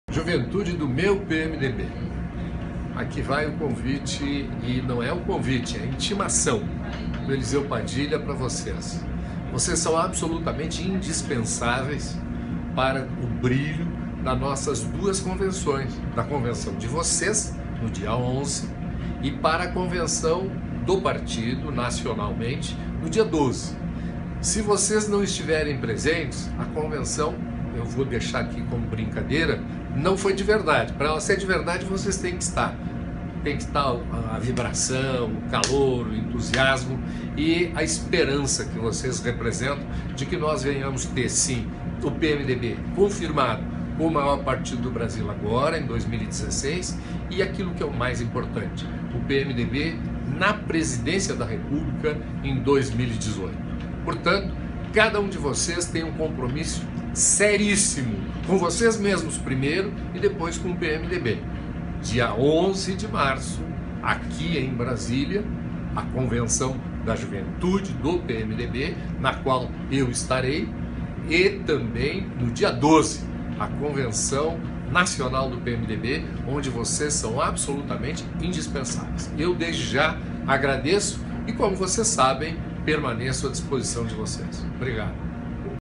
Eliseu Padilha convoca Juventude para participar da ‪‎Convenção Nacional‬ do ‪PMDB‬
Vice-presidente da Fundação Ulysses Guimarães Nacional, Eliseu Padilha, convoca juventude para participar da ‪Convenção Nacional‬ do ‪PMDB‬ dia 12 de março em Brasília.  Assista!

Confira, no link, as datas e locais das Convenções dos Núcleos do partido: goo.gl/1ctI6z